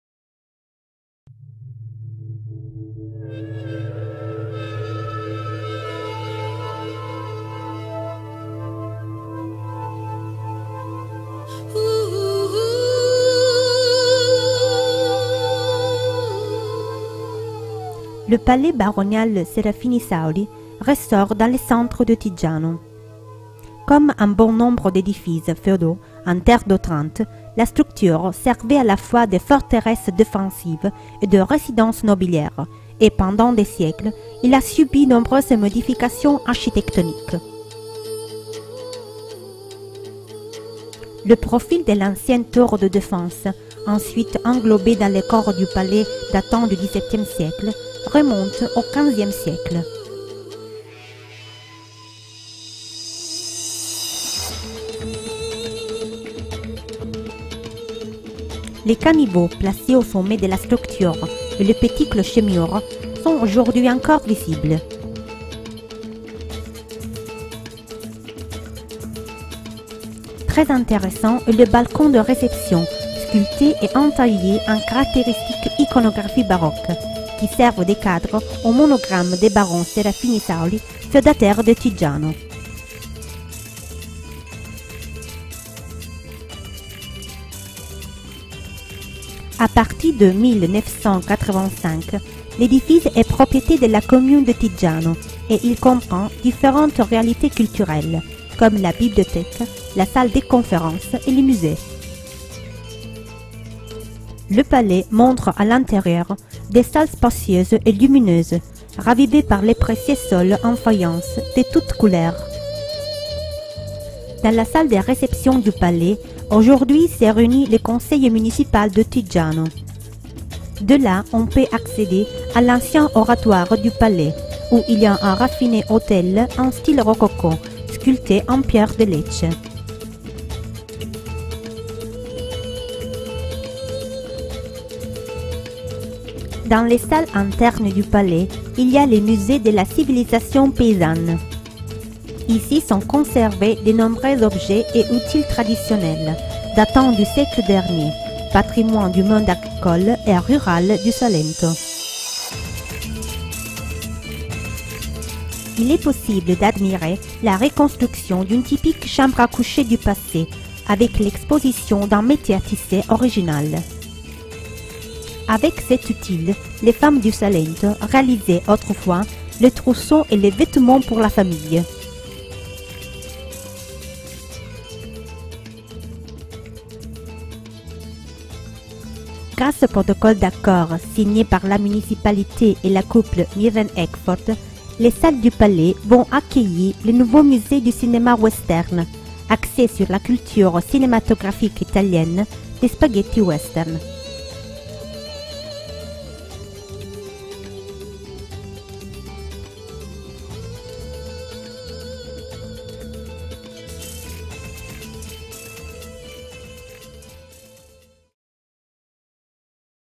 Ville de Tiggiano - Palais Baronnial
Le Palais Baronnial Serafini-Sauli ressort dans le centre de Tiggiano.
Comme un bon nombre d’édifices féodaux en Terre d’Otrante, la structure servait à la fois de forteresse défensive et de résidence nobiliaire et, pendant des siècles, il a subi nombreuses modifications architectoniques.
Le profile de l’ancienne tour de défense, ensuite englobée dans le corps du palais datant du XVIIème siècle, remonte au XVème siècle.